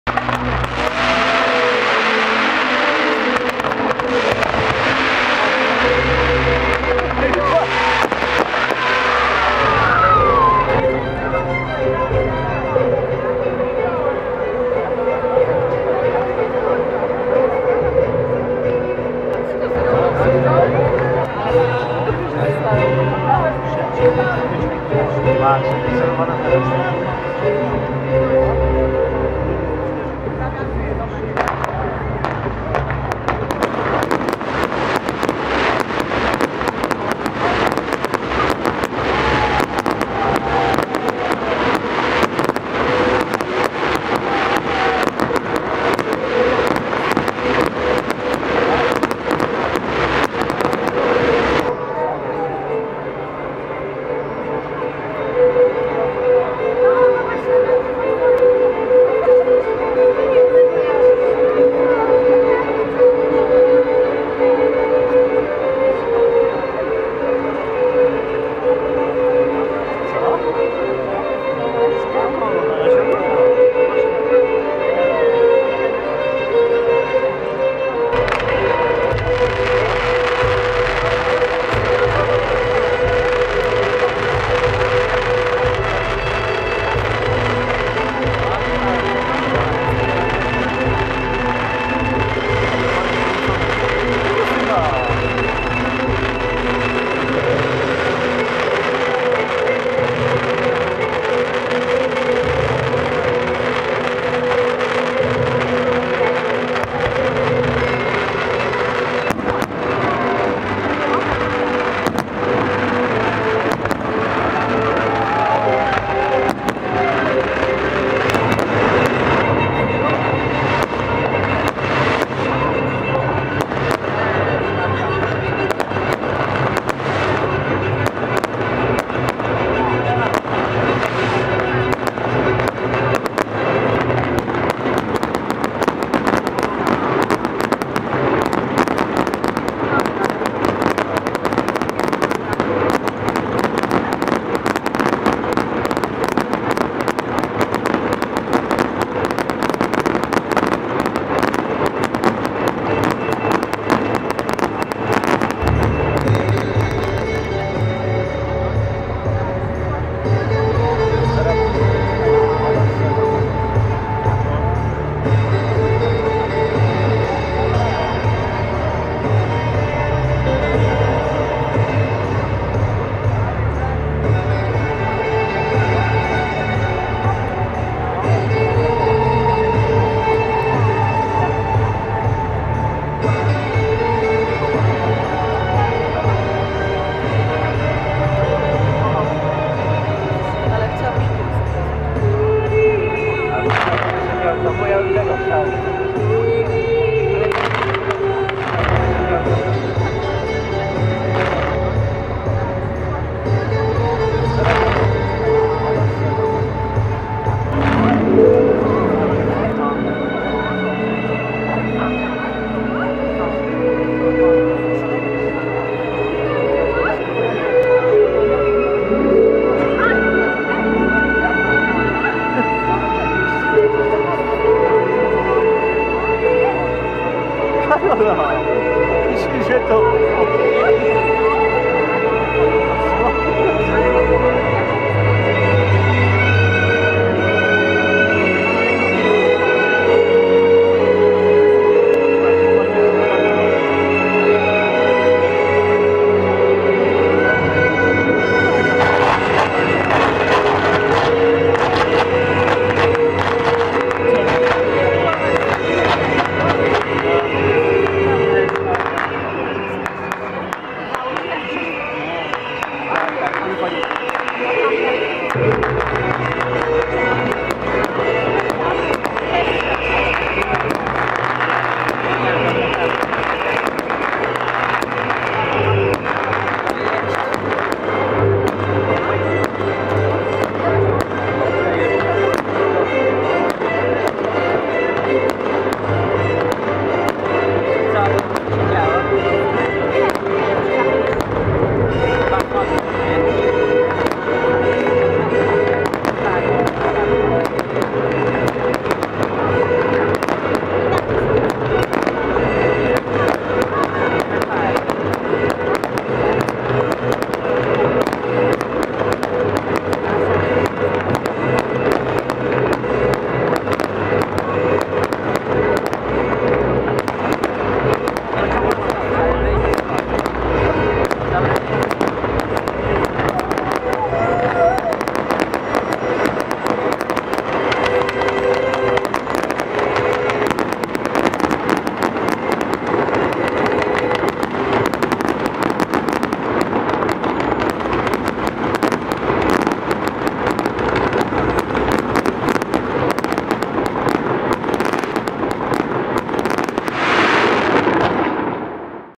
Great Dragon Parade (Wielka Parada Smoków)
The first event of Krakow's Great Dragon Parade is a combination of giant inflatable dragons, fireworks, lasers, water, and music on the Vistula River near the Wawel castle, produced by Theatr Groteska. This video is from the 16th annual parade in June 2016.